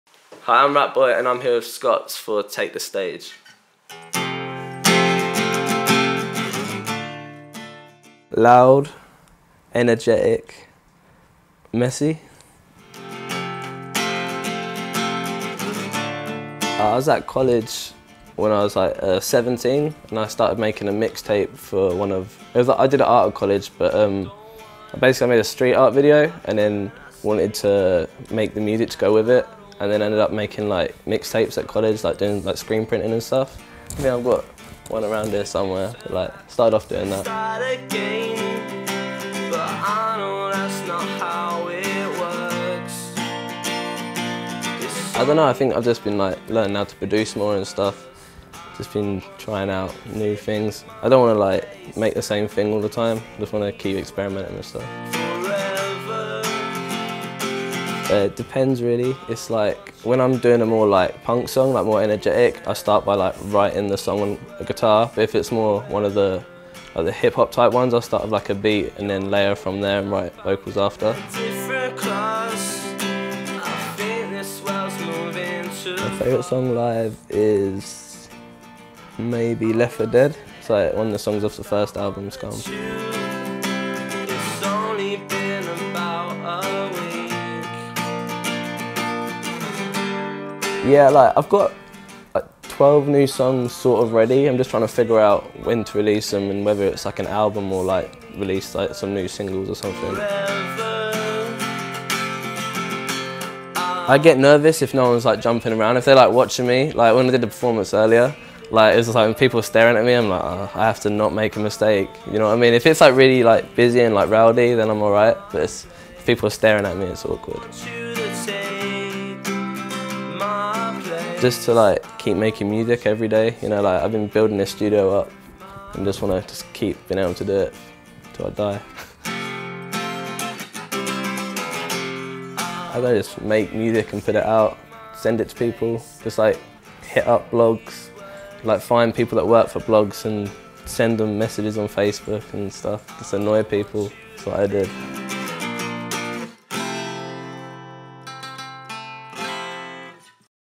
Take The Stage x Rat Boy - Exclusive Interview
Take The Stage; Volume 7!

Our Take The Stage campaign is smashing it and it's only going to get better! To recap, we've had Prose, Tom Grennan, The Hunna and now Rat Boy.

Trust us when we say, we've got a ton of legends lined up for you. But that's a secret... For now!

We recently travelled to hang out with the man himself. We got to chat with Rat Boy on a personal and professional level, we heard about how he started and the exciting things he's got lined up.